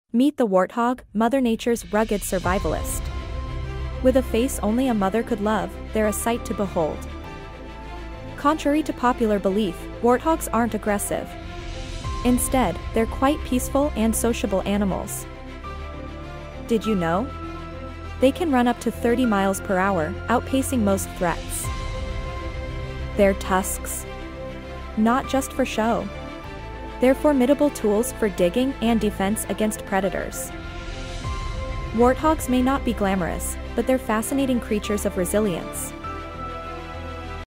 Warthogs 🐗 How Dangerous Are They?
Warthogs Are Viewed As Dangerous Creatures, But Are They Really? Check Out This Short For This And Other Interesting Facts About Nature's Bulldozer.